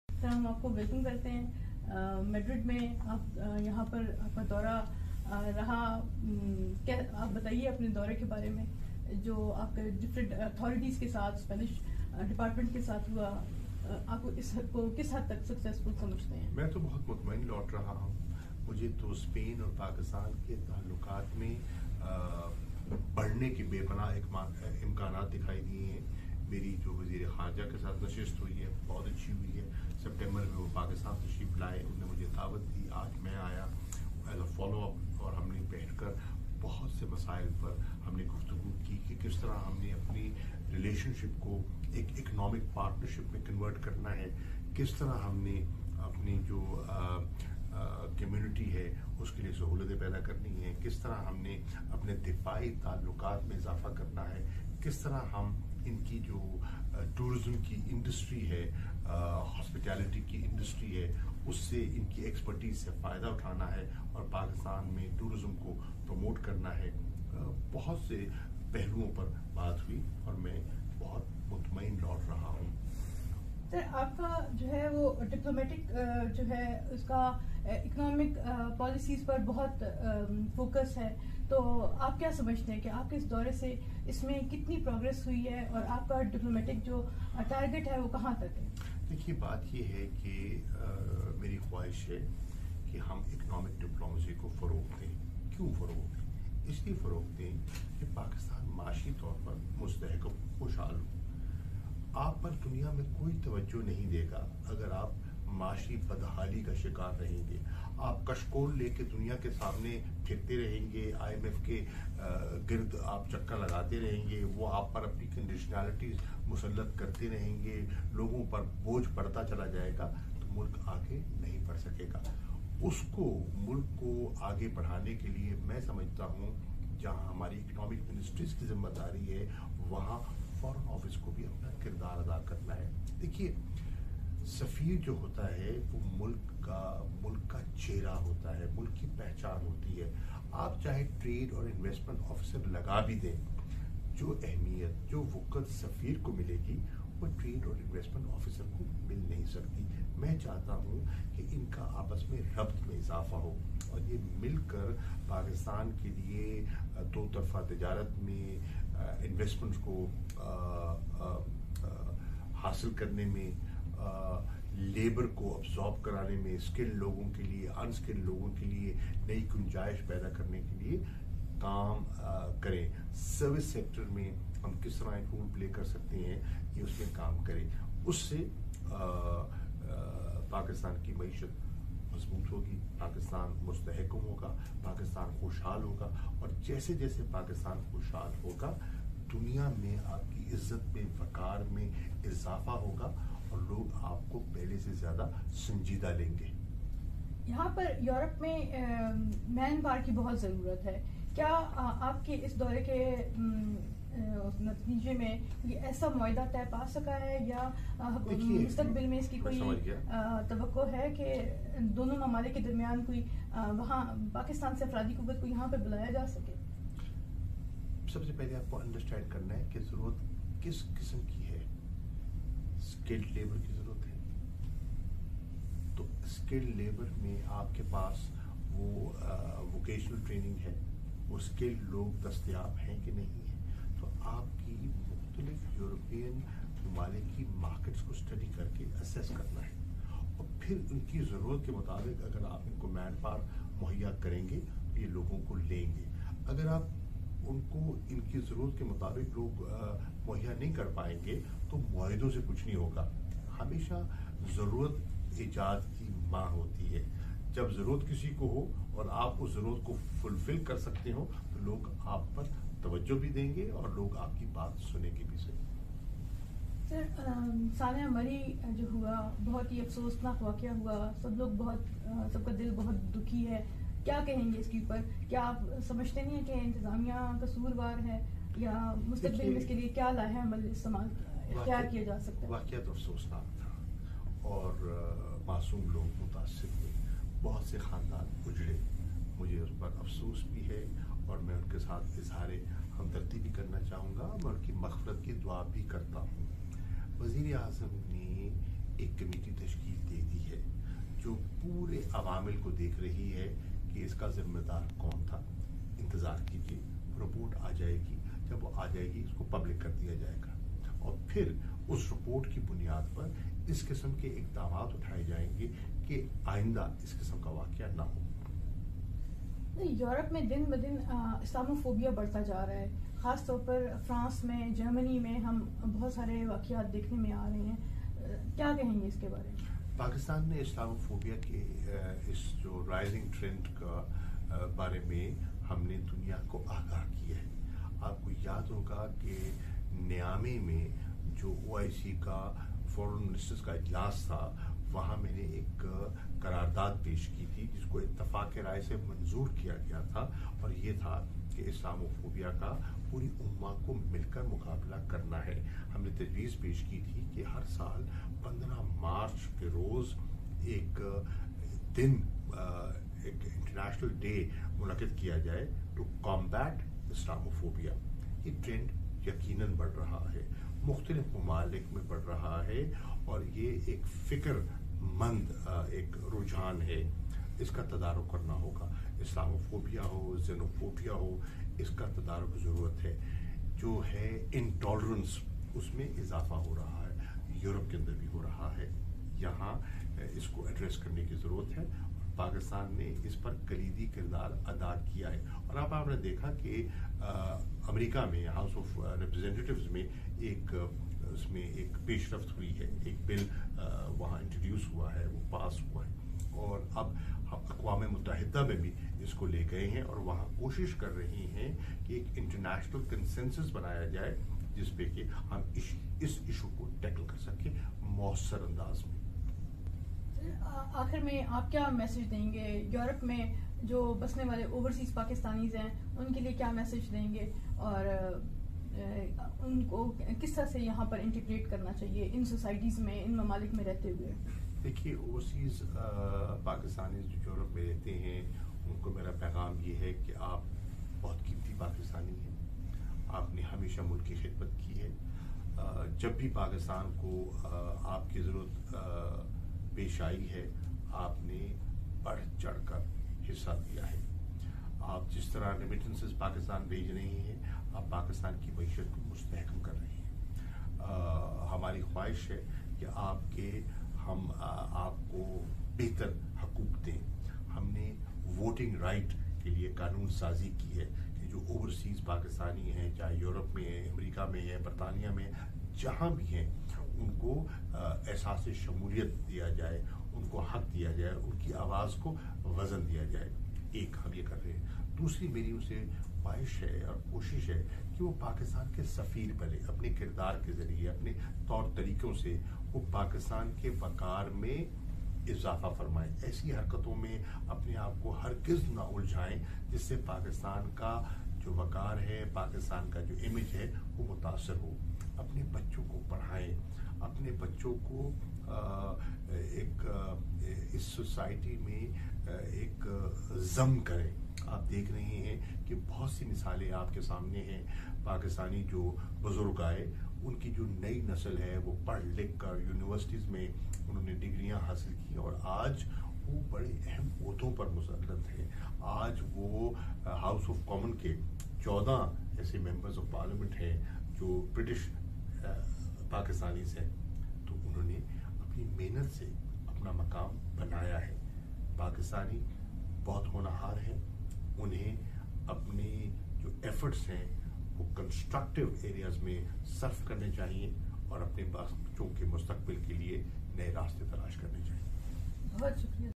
Madrid: Minister of Foreign Affairs of Pakistan Shah Mahmood Qureshi talks to local MediaJournalists
Madrid: Minister of Foreign Affairs of Pakistan Shah Mahmood Qureshi talks to local Media Journalists
12 January, 2022

وزیر خارجہ مخدوم شاہ محمود قریشی کی دورہ ء اسپین کے حوالے سے میڈرڈ میں صحافیوں سے گفتگو